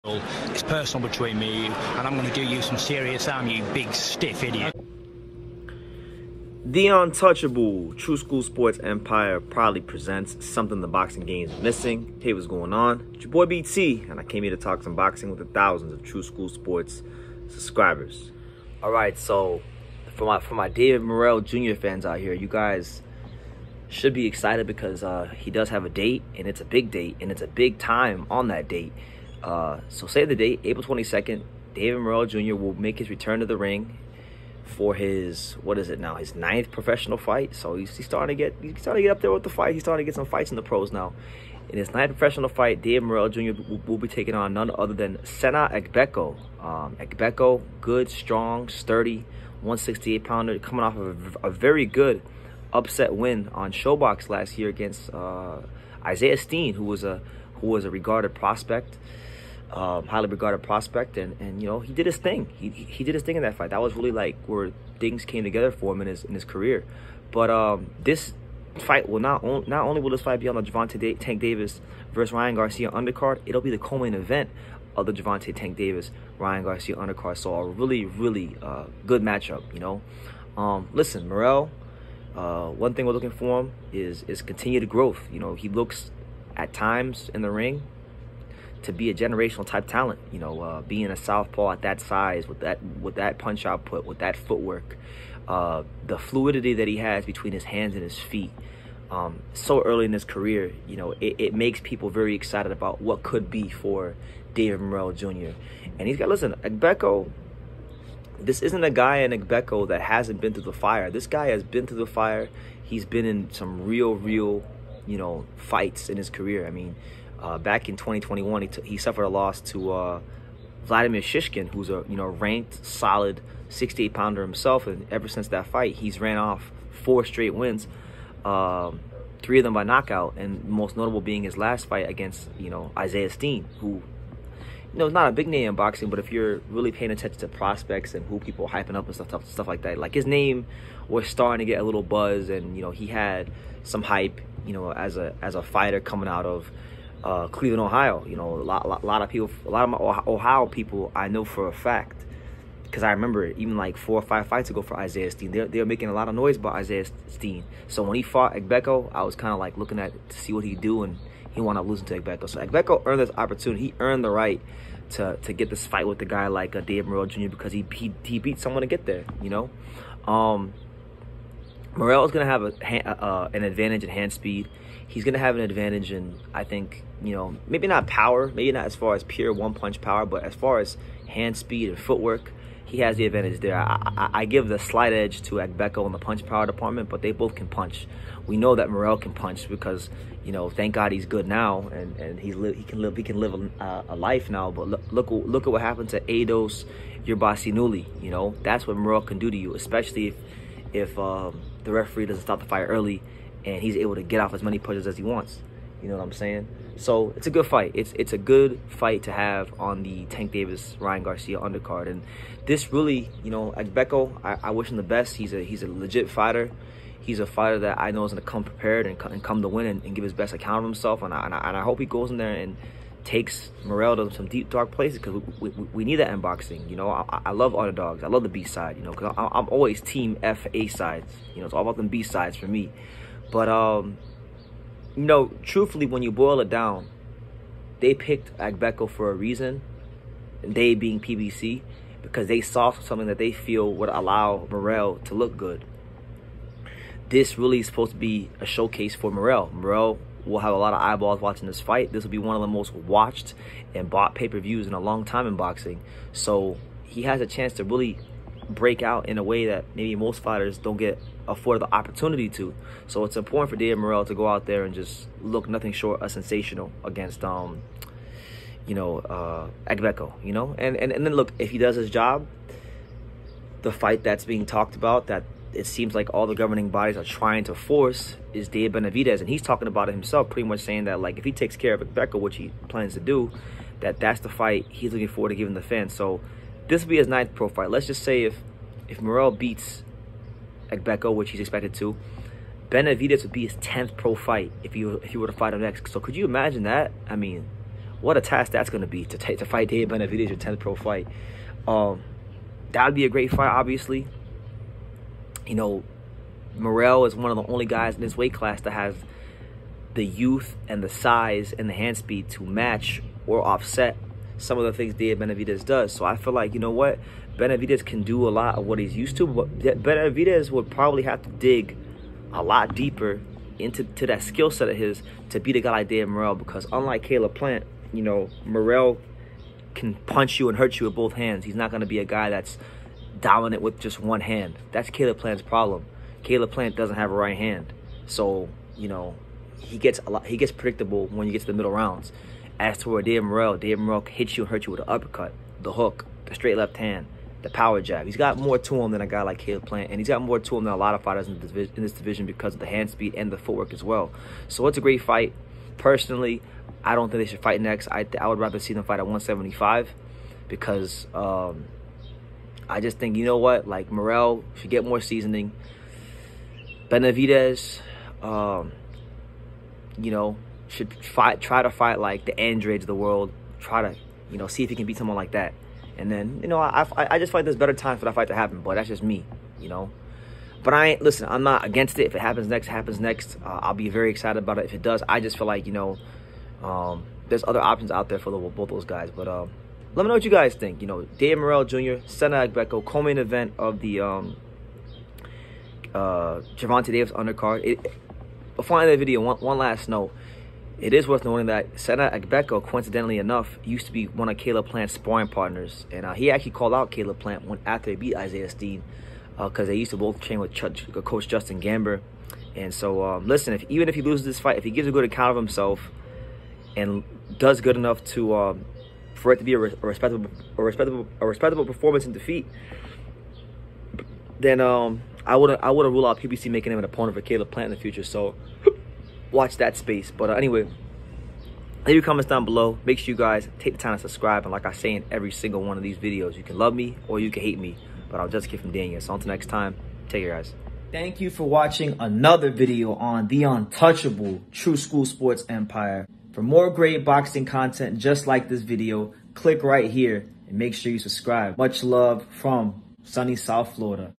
DAVID MORRELL JR VS SENA AGBEKO- A REAL TEST BEFORE HE'S READY FOR DAVID BENAVIDEZ
This a proper fight for David Morrell Jr at this stage of his career and a chance to really make an impression as co-main event of Gervonta Davis vs Ryan Garcia.